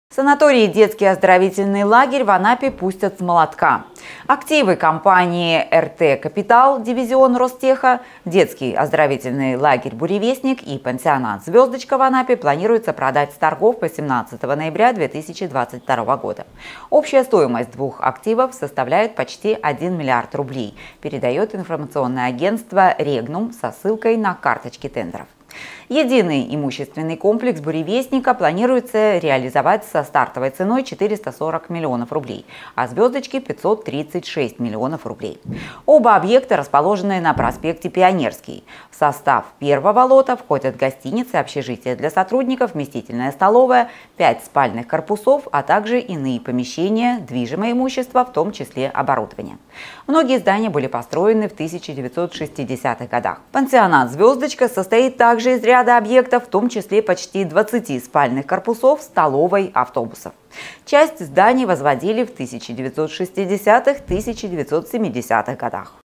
Санаторий и детский оздоровительный лагерь в Анапе пустят с молотка
Санаторий и детский оздоровительный лагерь в Анапе пустят с молотка.  Активы компании «РТ-Капитал» (дивизион Ростеха) — детский оздоровительный лагерь «Буревестник» и пансионат «Звёздочка» в Анапе планируется продать с торгов 18 ноября 2022 года. Общая стоимость двух активов составляет почти 1 млрд рублей, передаёт ИА REGNUM со ссылкой на карточки тендеров